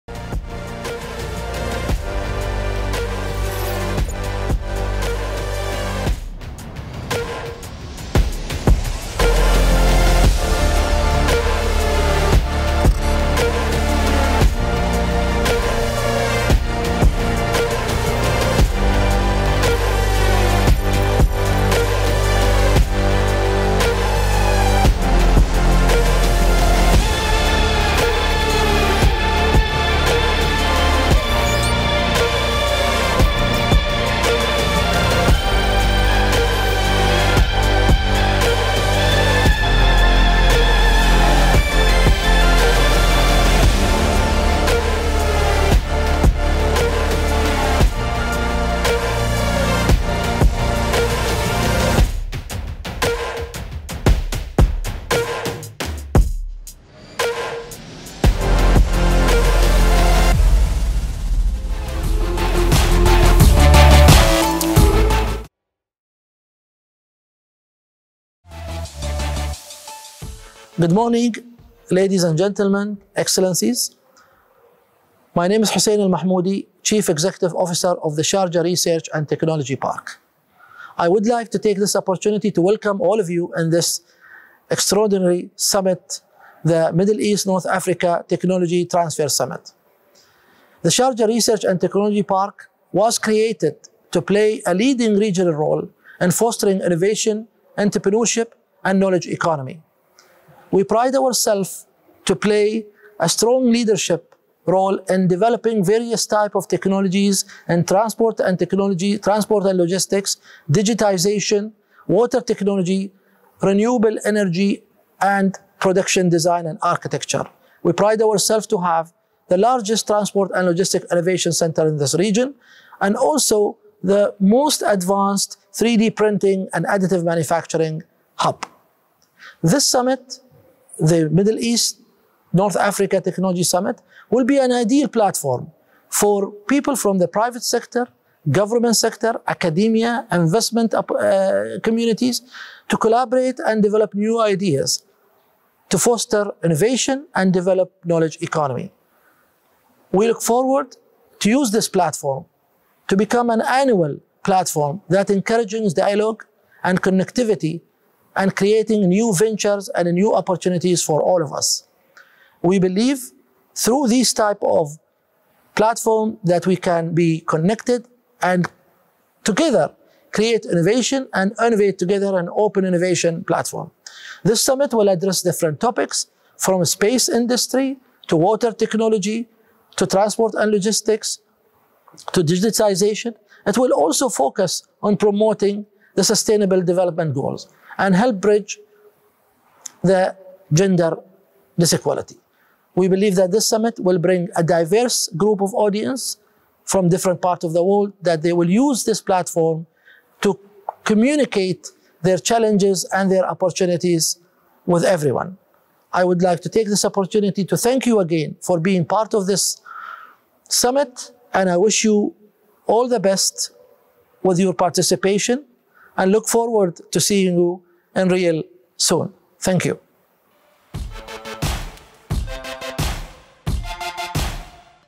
Grand opening of the MENA Innovation Technology Transfer Summit (MITT Summit)
The Sharjah Research Technology and Innovation Park (SRTI Park), UAE’s premier research and innovation hub, organized the first edition of the M.E.N.A. Innovation Technology Transfer Summit (MITT Summit) on September 22nd, 2020. The Summit brought together global investors, government and private sector representatives, experts, entrepreneurs, academics, and other relevant stakeholders for an immersive experience of knowledge sharing, business showcasing and networking.